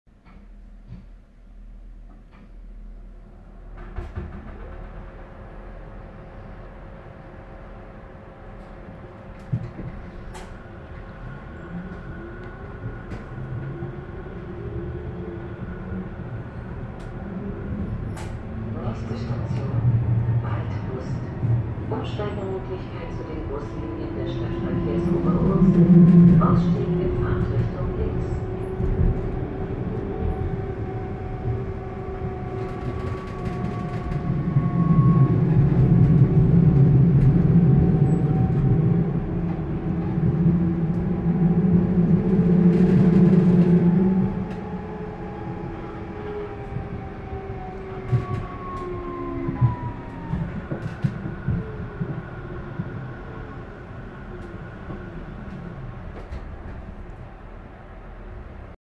VGF U-Bagn Typ U5 50 Linie U3 Mitfahrt Hohemark - Waldlust
In diesem Video fahren wir mit der U-Bahn Typ U5 50 der Verkehrsgesellschaft Frankfurt als Linie U3 von Oberursel Hohemark nach Waldlust.

Aufgenommen mit dem Samsung Galaxy S7 Edge.